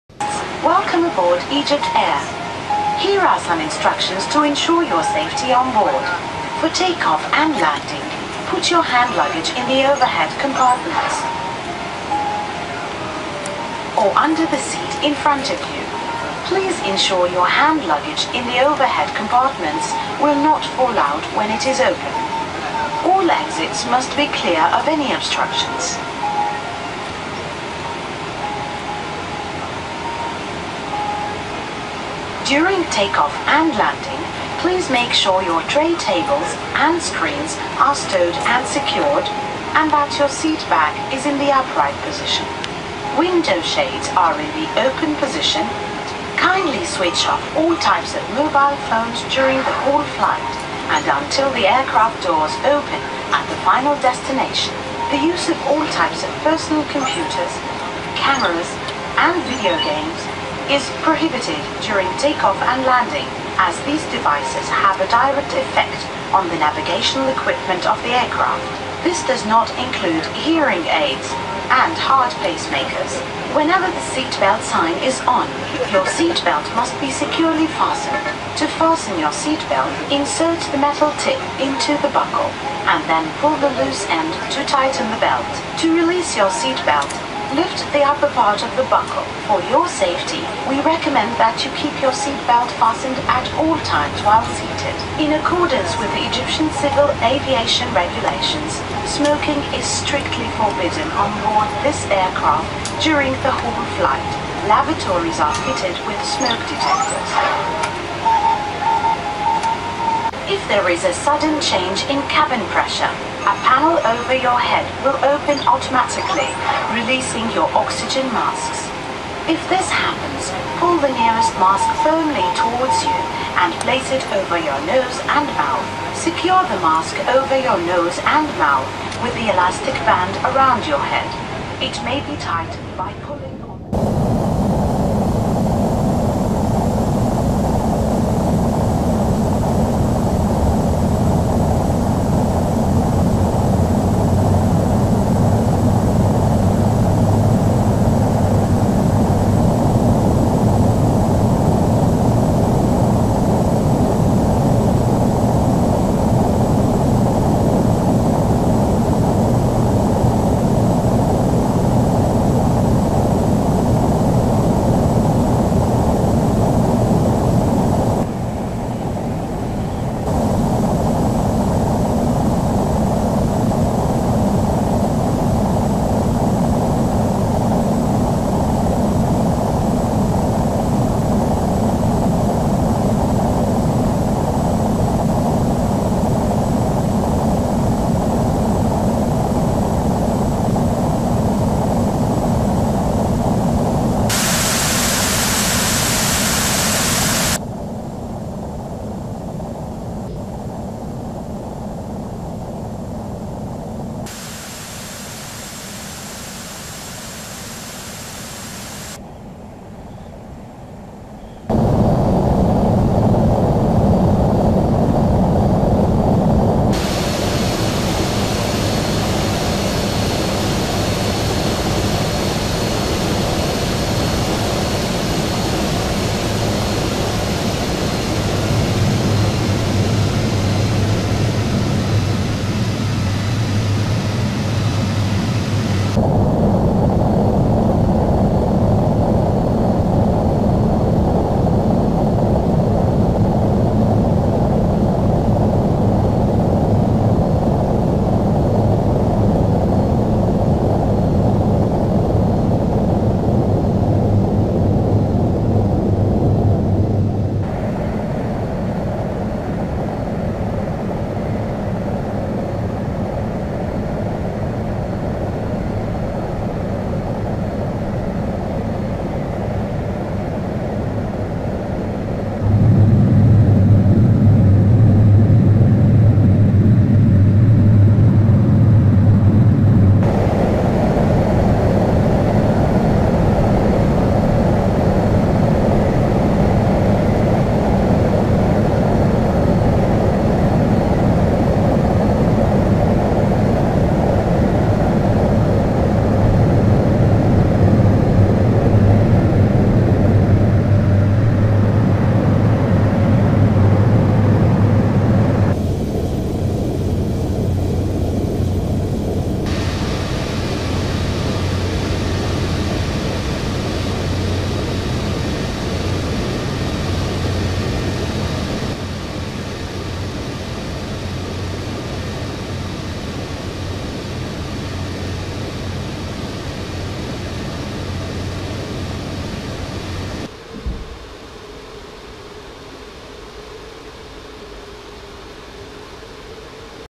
A320 EGYPTAIR CAIRO - ALGIERS
A320 EGYPTAIR CAIRO - ALGIERS as MS 845, flight time: 3hs 57min.